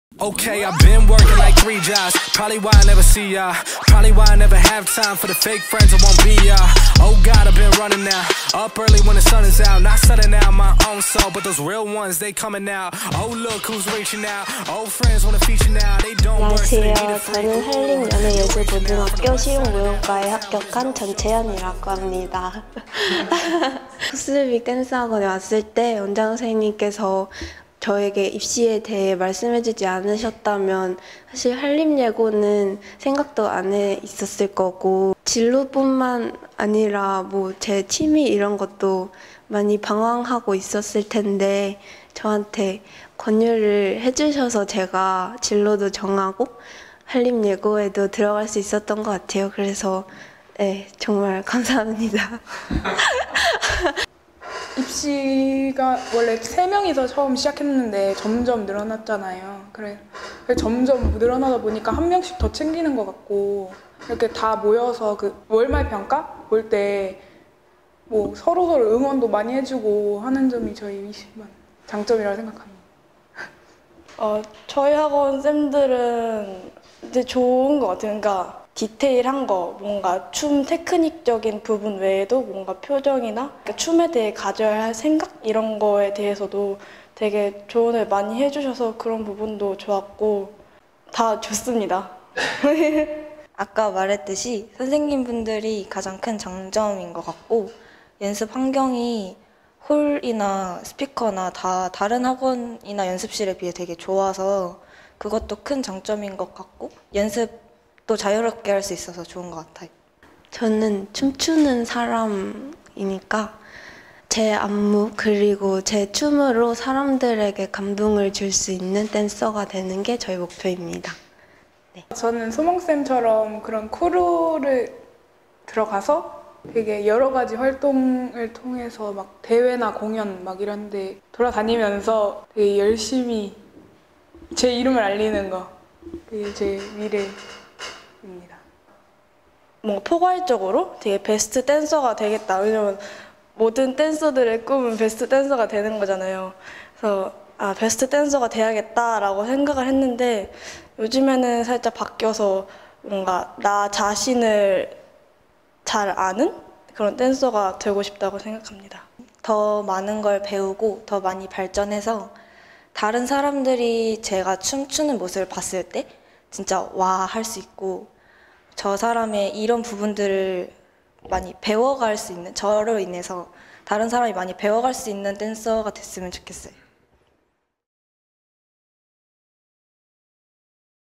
[동탄댄스학원] 코스믹댄스 입시반 인터뷰
코스믹댄스학원의 자랑!
입시반 친구들의 인터뷰 영상입니다:)
어떤 이야기를 했을지 귀기울여 들어주세요-